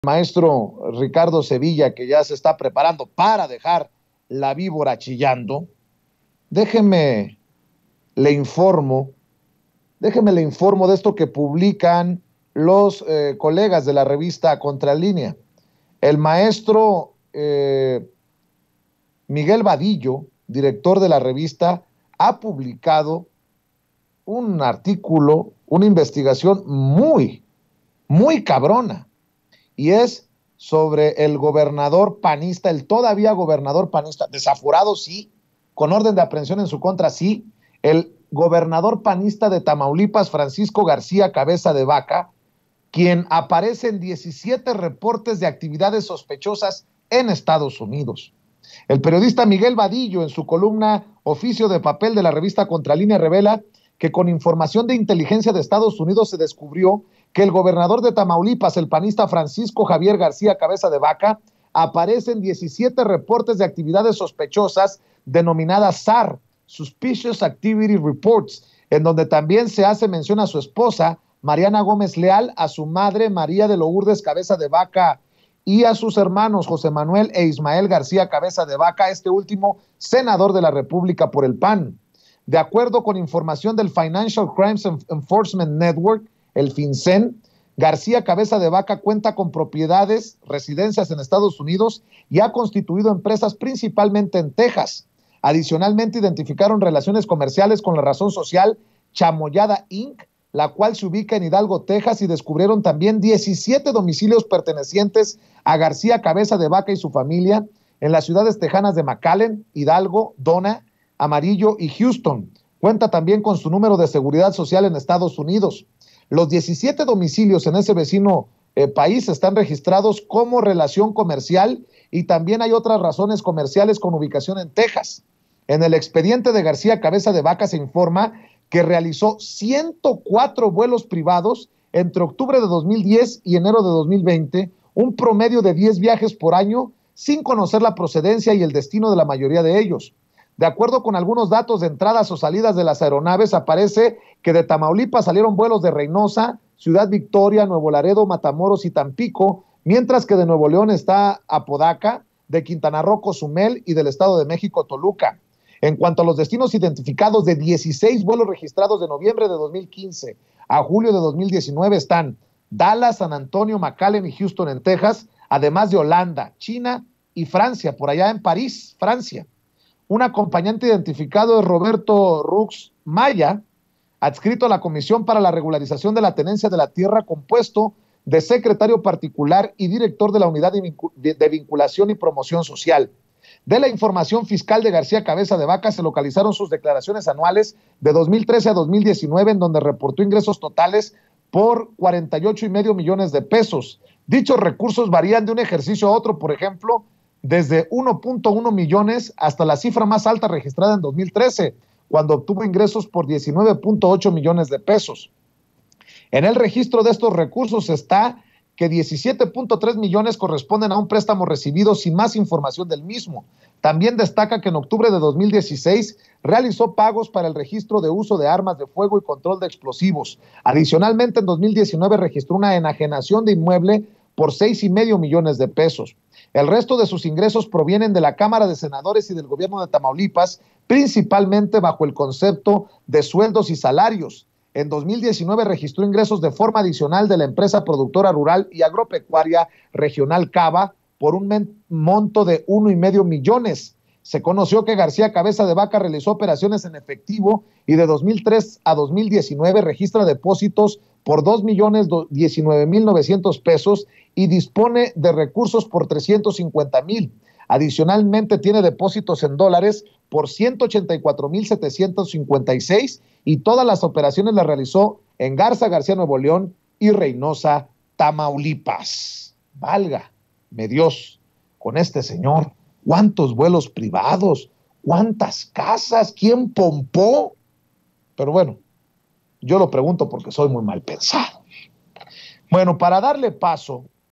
MIGUEL BADILLO EXHIBE QUE CABEZA DE VACA APARECE EN 17 REPORTES DE ACT. SOSPECHOSAS EN EEUU
El maestro Miguel Badillo publica en Contralínea una investigación muy fuerte sobre el todavía gobernador panista de Tamaulipas Francisco García Cabeza de Vaca, de quien asegura aparece en 17 reportes de actividades sospechosas en Estados Unidos.